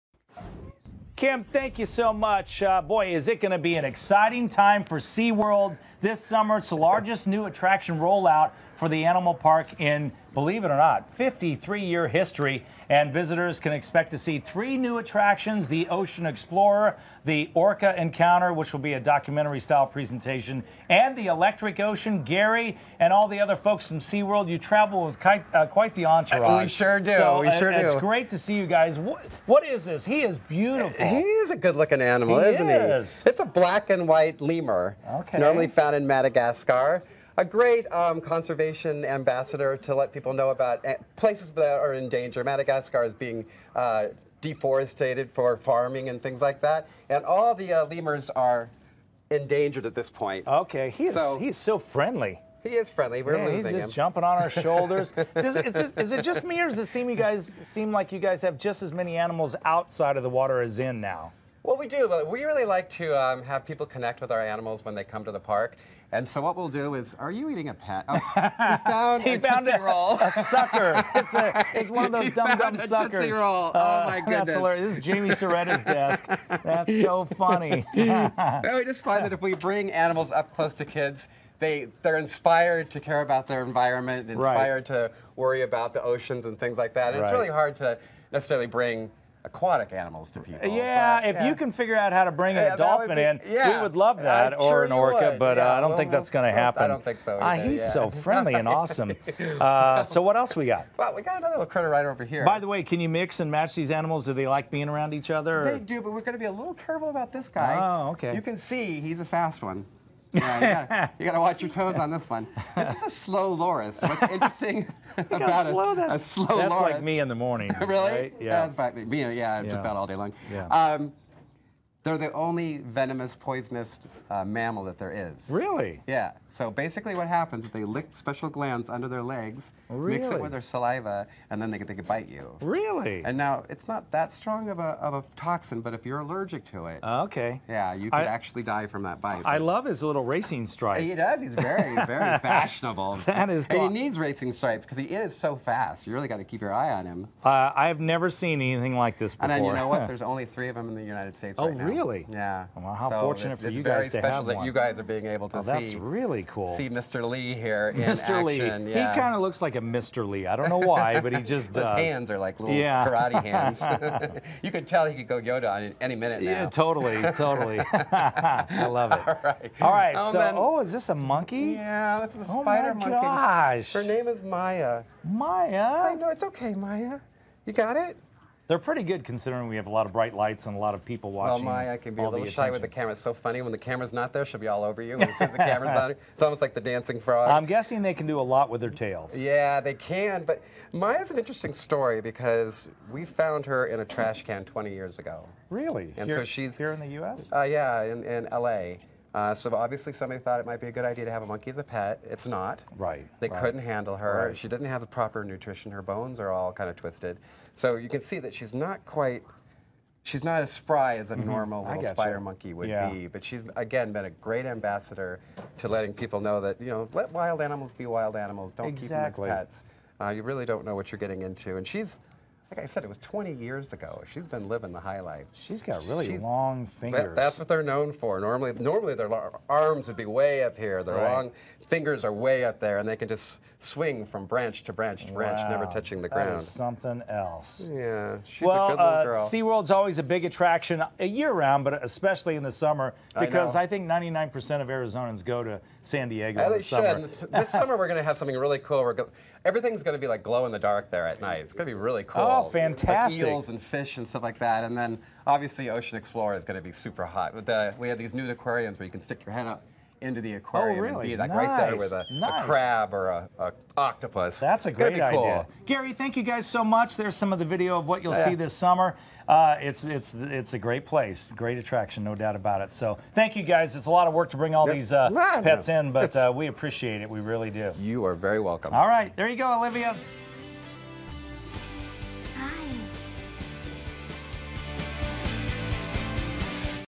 SeaWorld San Diego rolls out new attractions (part 2)
SeaWorld joins us with animals as this is the largest new attraction roll-out for the animal park in its 53-year history. Visitors can expect to see three new attractions this summer, the Ocean Explorer, the Orca Encounter which will be a documentary-style presentation, and the Electric Ocean.